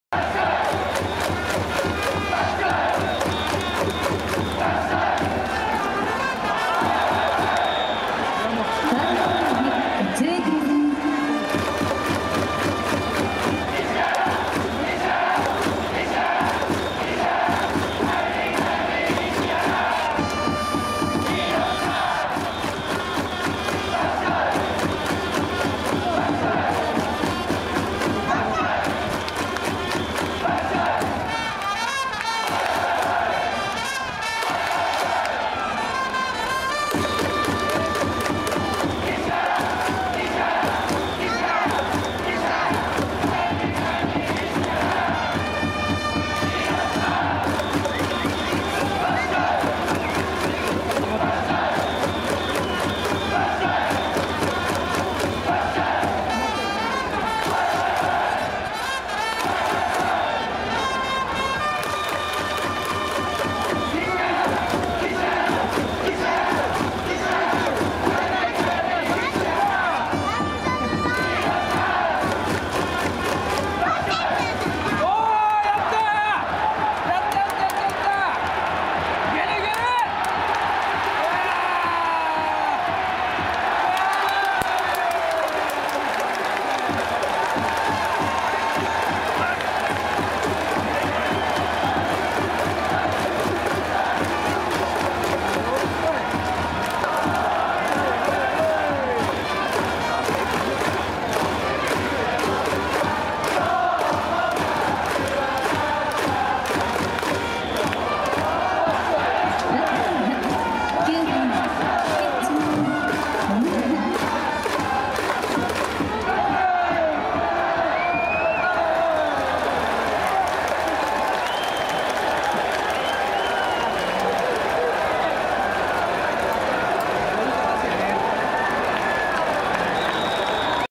広島カープ応援シーン（2013年横浜DeNAベイスターズ10回戦）①
2013年7月9日横浜スタジアムで行われたプロ野球公式横浜DeNAベイスターズ対広島東洋カープ第10回戦での広島の応援シーン